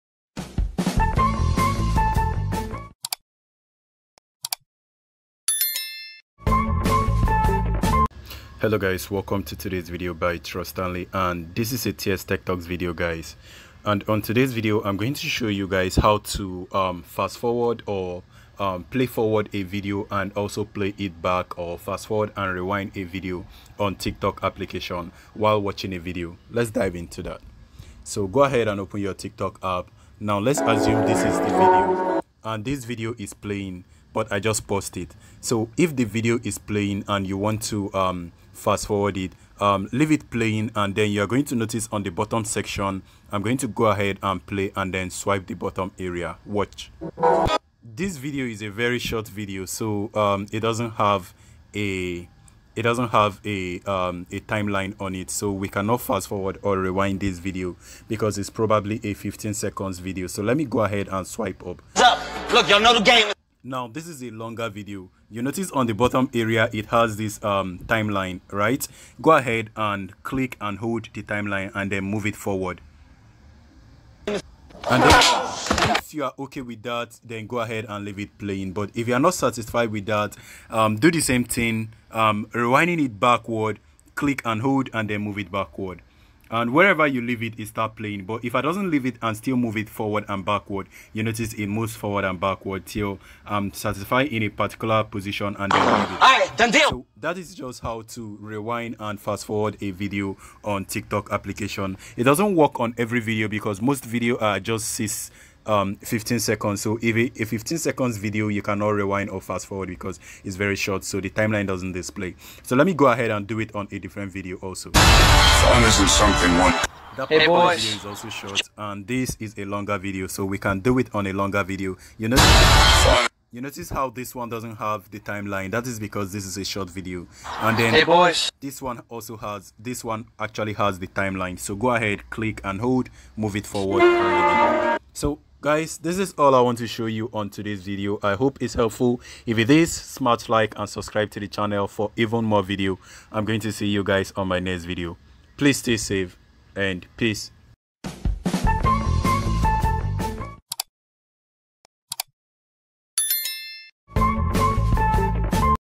How to fast forward and rewind a video on TikTok app
Even though TikTok videos are just a few minutes, you might still want to fast forward or rewind them. TikTok has a progress bar so you can skip through them; however, it's not available for all videos, its only available for long videos any video below 15 seconds will not have this option